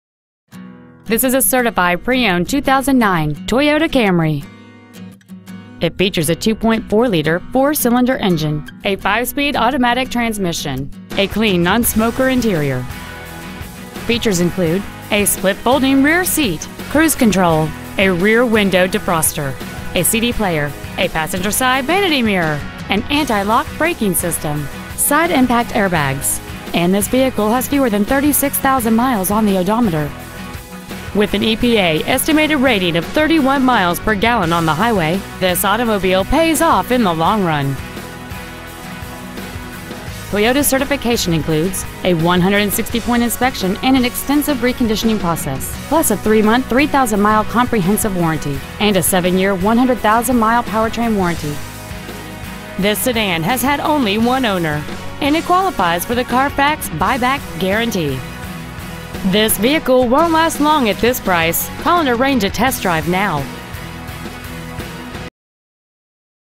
Certified 2009 Toyota Camry Concord CA 94520
Year : 2009
 Make : Toyota
 Model : Camry
 Engine : 2.4L I-4 cyl
 Trans . : 5-Speed Automatic
 Exterior : Gray
 Miles : 35,946
 Interior : Gray
 Concord Toyota
 2009 Toyota Camry Concord CA